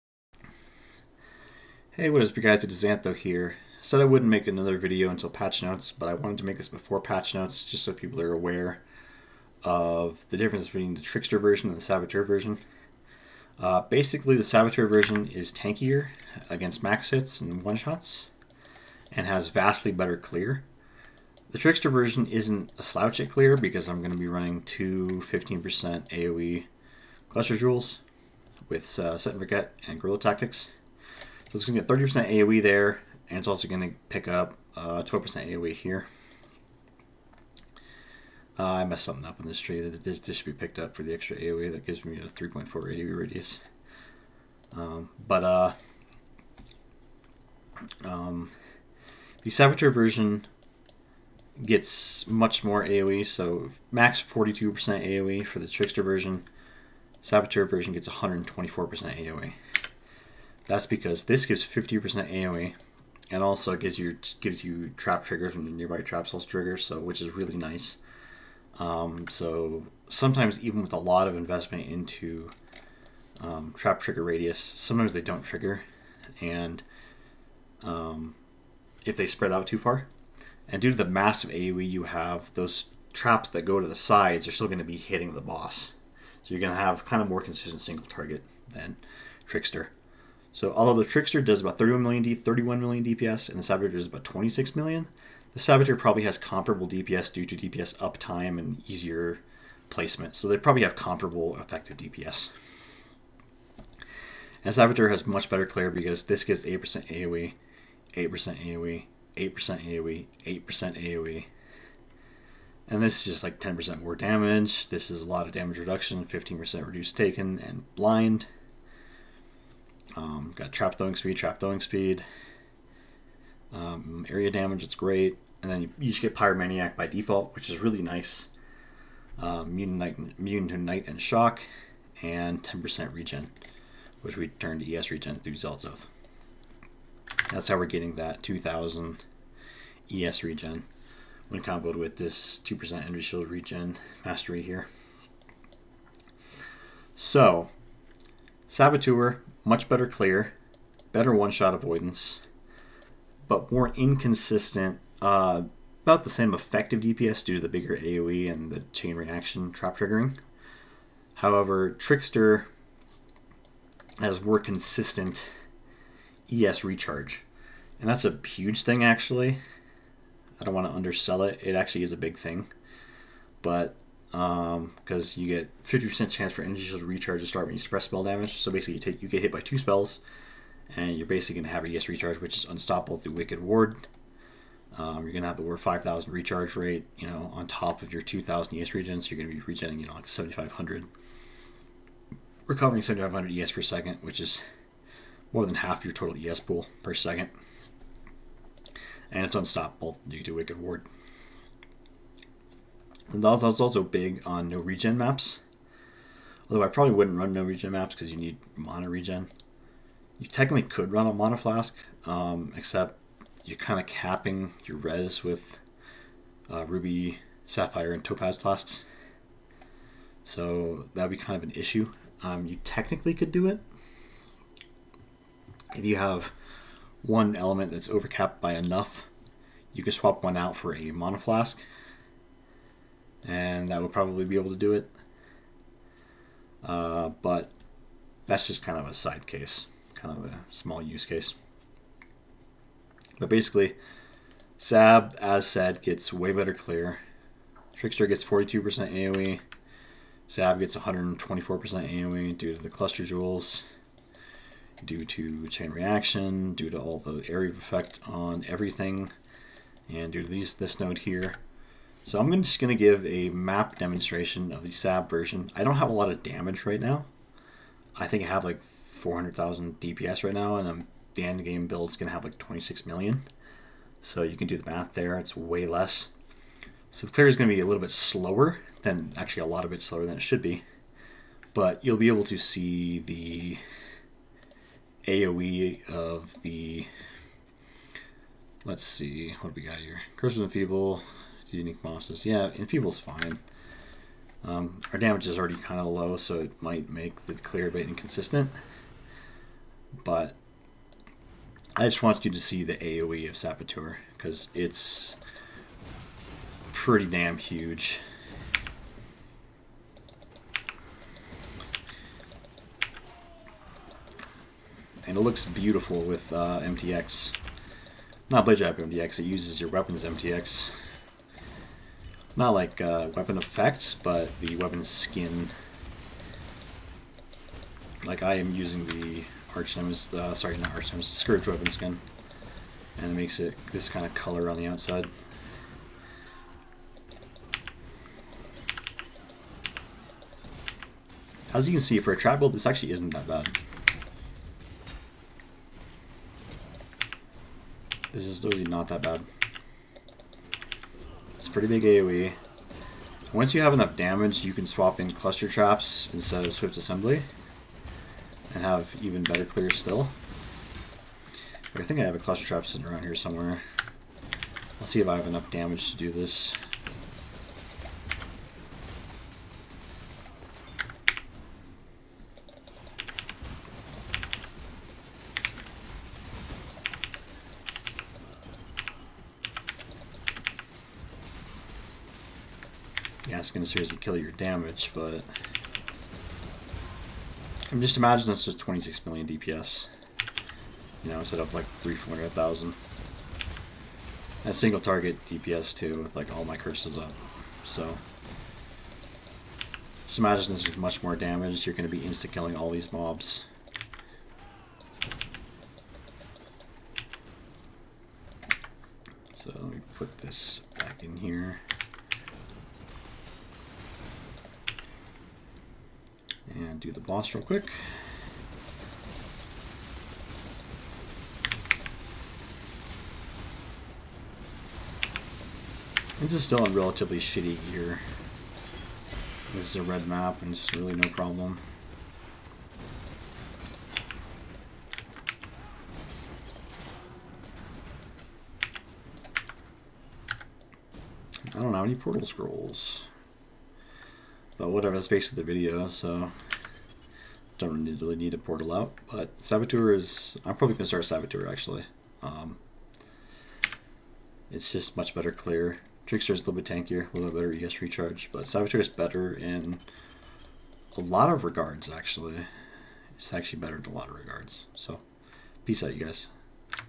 Trickster vs. Sab for ES Blade Trap + Sab clear example on terrible early-league gear.
Well I accidentally lied last video and ending up making another video. Just wanted to get some of the Trickster vs. Saboteur information out there, and give an example of high AoE Saboteur clear (with mostly crappy gear and about 1/50th of endgame DPS). I will likely be leaguestarting Saboteur due to the far better clear and quicker access to Pyromaniac.